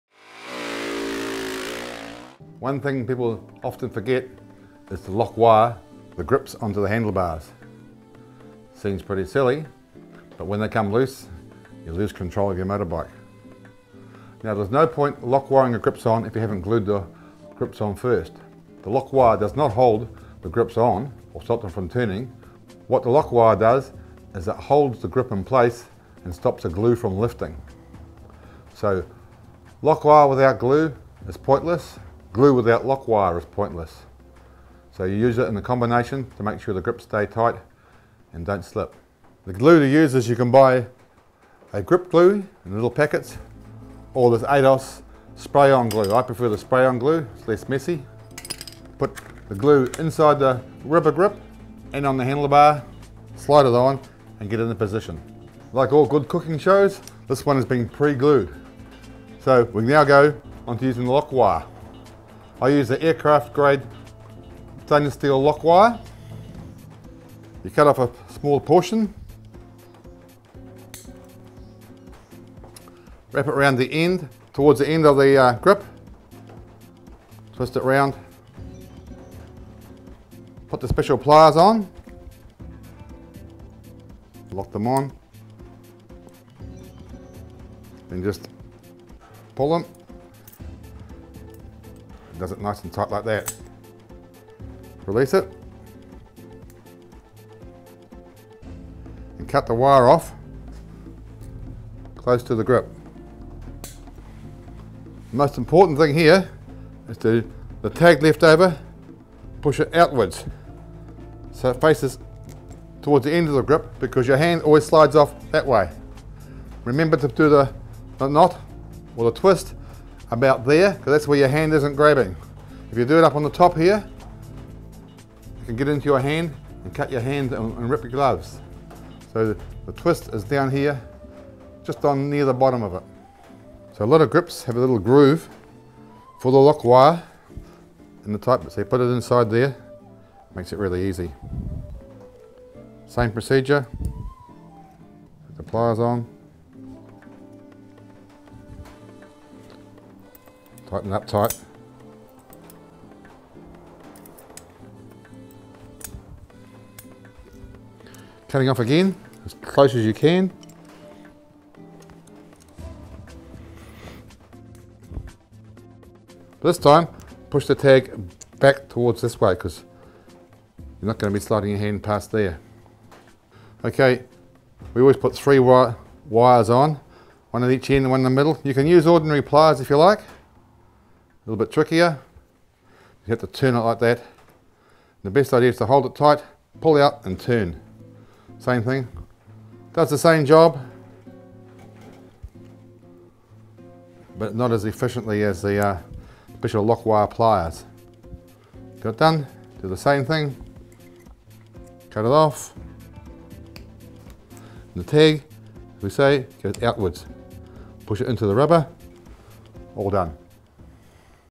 How To Lockwire Handgrips
Greg from Power Adventures demonstrates how to lockwire your bike's handgrips.